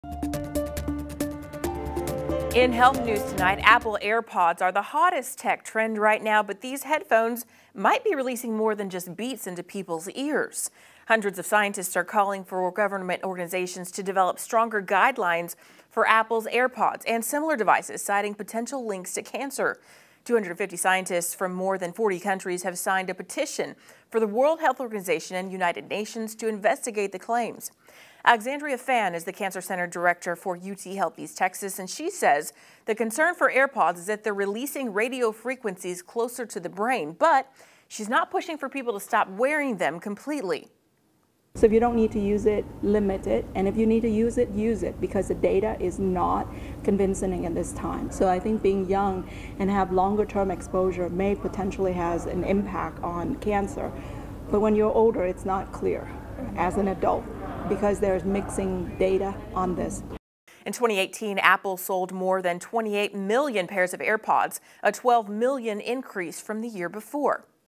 ETX doctor gives opinion on whether Apple's Airpods can cause cancer
Hundreds of scientists are calling for government organizations to develop stronger guidelines for Apple's Airpods and similar devices citing potential links to cancer.